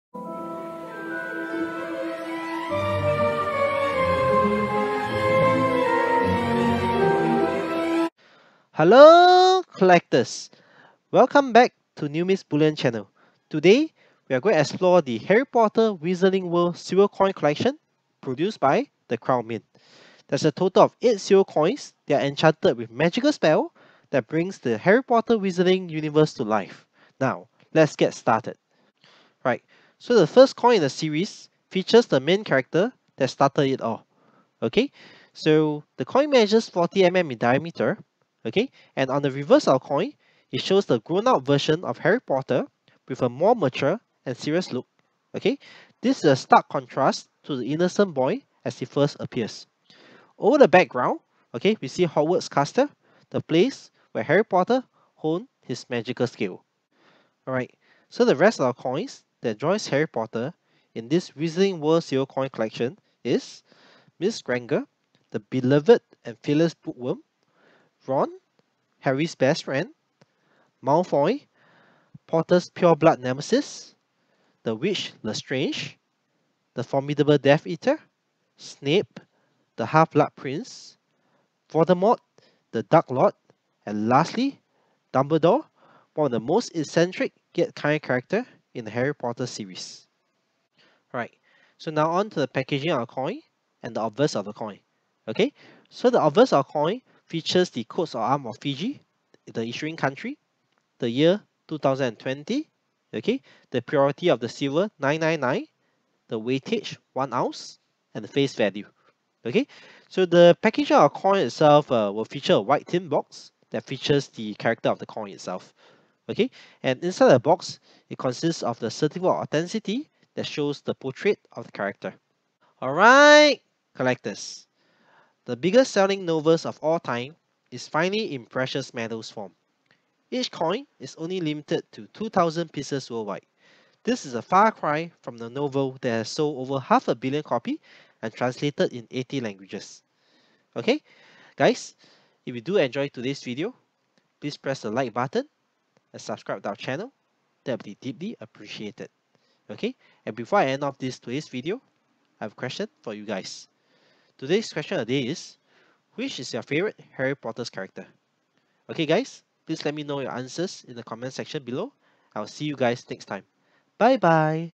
Harry Potter: The Wizarding World
Today, we explore the Harry Potter Wizarding World Silver Coin Collection.

📰 Coin Info